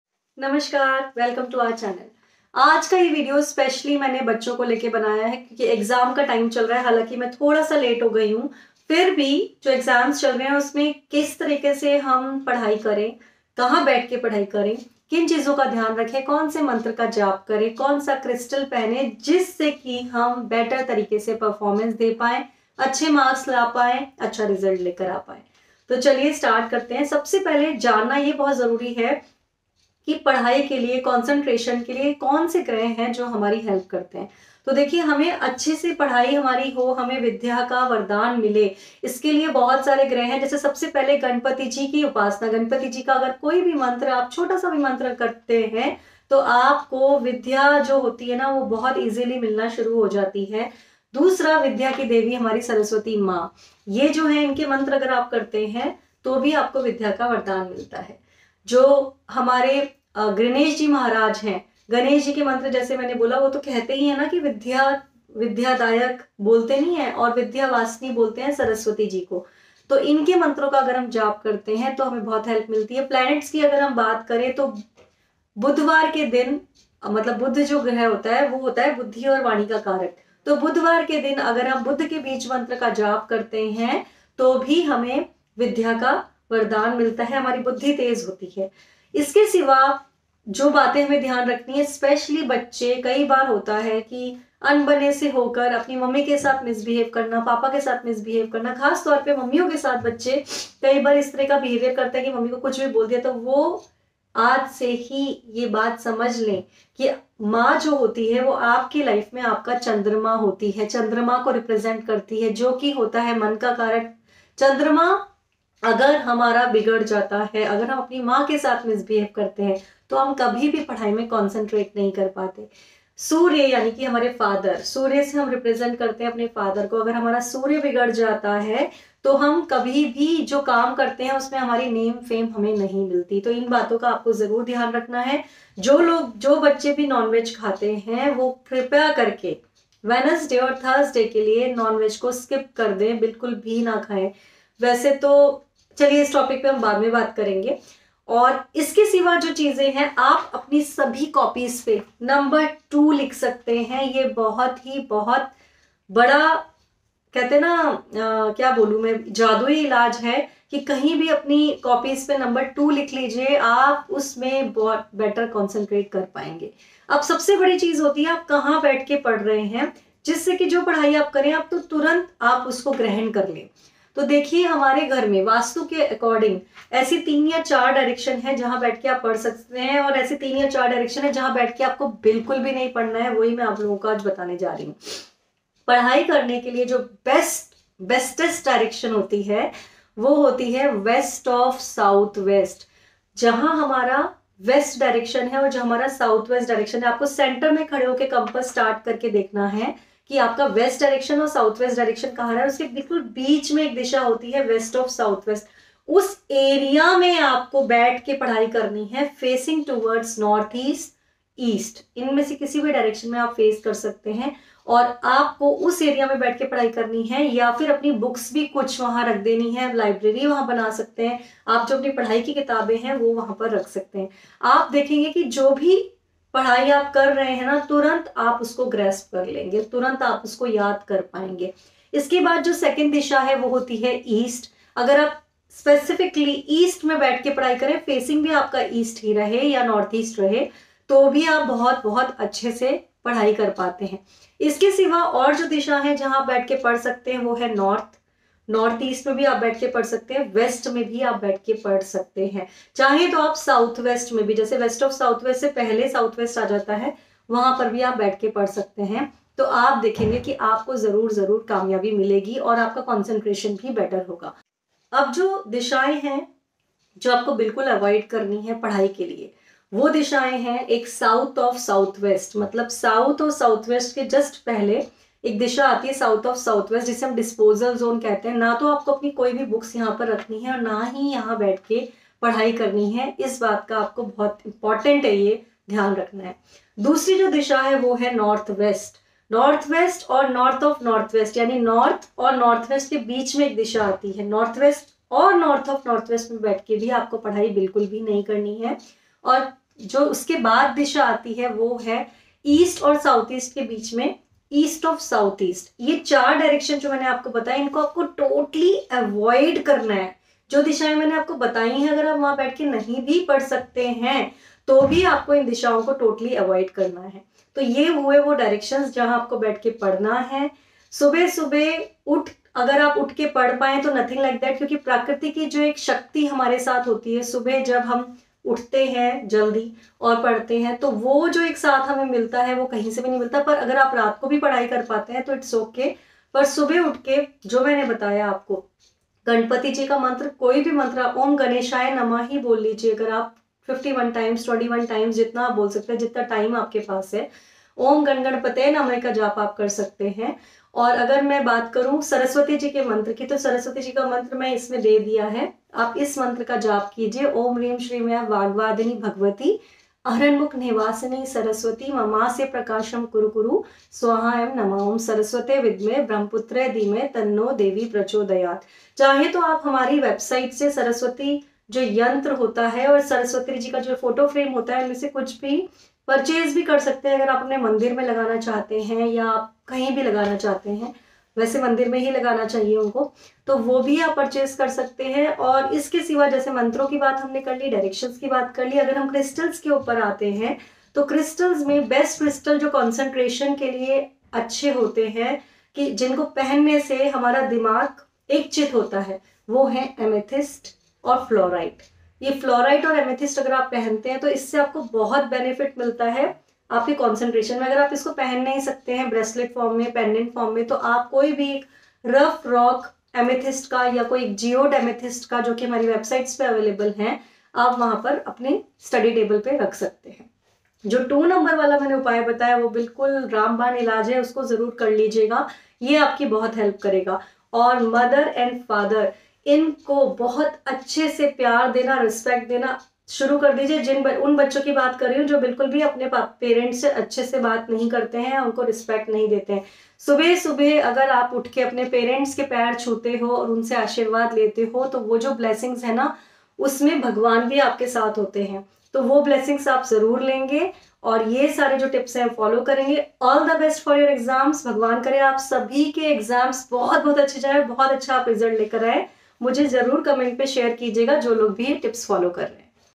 Special Tips For Exams To Score Good Marks | Vaastu Nidhie
Special Tips For Exams To Score Good Marks |Tips For Exams

गणेश जी के मंत्र:
१-ओम गणेशाय नमः
२ ओम् गन गणपते नमः
सरस्वती जी के मंत्र:
१ओम रीम श्रीम एम वाघवादिनी भगवती अरहन मुख निवासिनी सरस्वती ममासे प्रकाशम कुरुकुरू स्वाहा एम नमः
२-ओम सरस्वती विदमहे ब्रह्मपुत्र धीमहे तन्नो देवी प्रचोदयात।